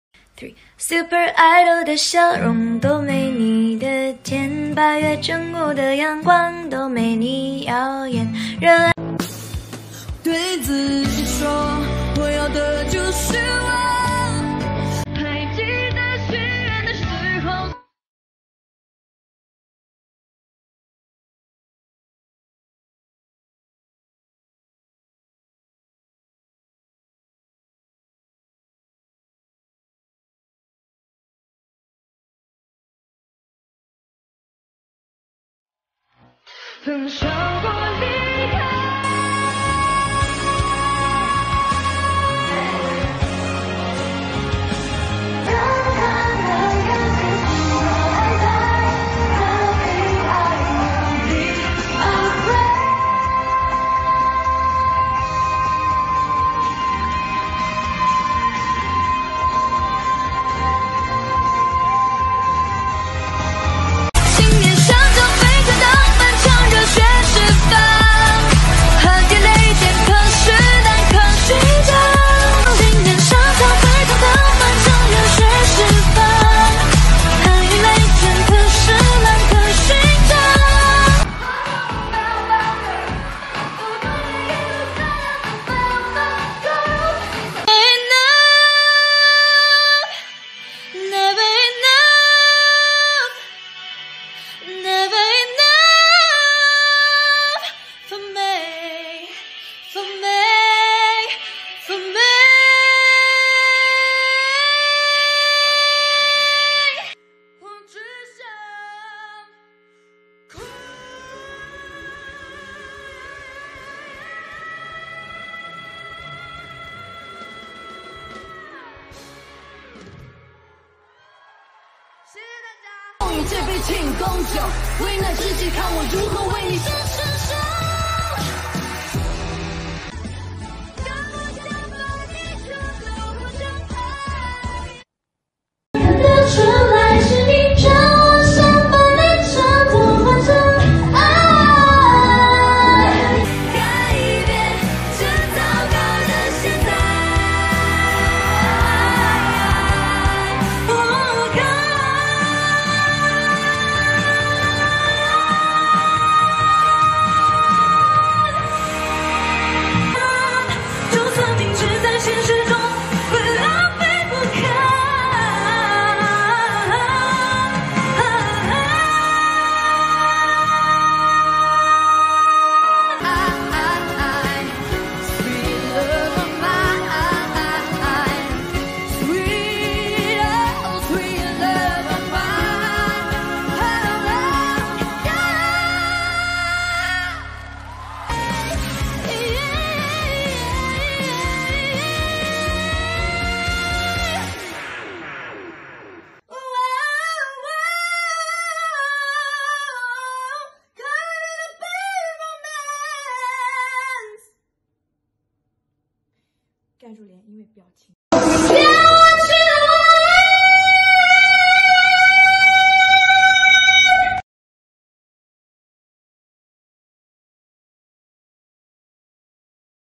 希林娜依高和陈卓璇在同音域的表现比对，没想到陈卓璇实力秒杀希林
两位女团成员在同音域上的表现对比。已经是2021年了，之前的音域视频已经过时，用了2人的新素材。

针对视频的简单说明：
1. 尽量选择2家粉丝的直拍/录音视频；
2. 部分素材直接取自粉丝的成品素材（陈素材是我本人提供的，5素材是其他粉丝提供）；
3. 有一部分缺乏高质量素材对比而被舍弃（比如陈卓璇的F#5就因为缺乏对比而被弃用）